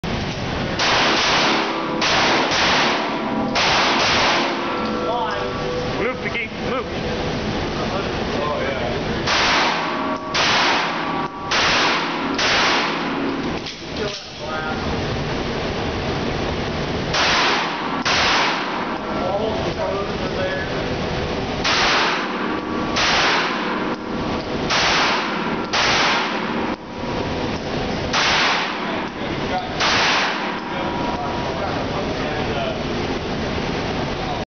IDPA 1/13/11 ~ CoF #3 ~ Shooter #3
Double Tap Defensive Shooting Club (DTDSC) IDPA on 1/13/11 at Kaw Valley Gun Club, Topeka, KS

Shooter #3 is the shooter and Keith has the shot timer.

CoF: Begin about 5 yards from the threat target, facing up range with hands at your sides. At the buzzer, turn, draw and fire 2 rounds from retention at the threat target directly in front of you, then while retreating to high cover, engage the remaining 2 threat targets with 2 rounds each in tactical priority (near to far). From one side, fire 2 rounds each into each of the 3 threat targets in tactical priority (slicing the pie); move to the other side of the high cover and engage the same 3 threat targets with 2 more rounds in tactical priority (slicing the pie).
All 3 threat targets should have 6 hits each; minimum rounds per shooter: 18